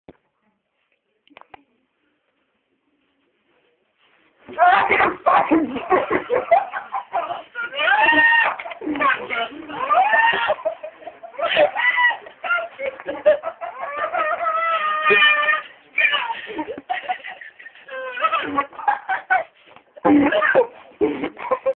nightmare on east view close
james dresses up as freddy krueger and scares his mum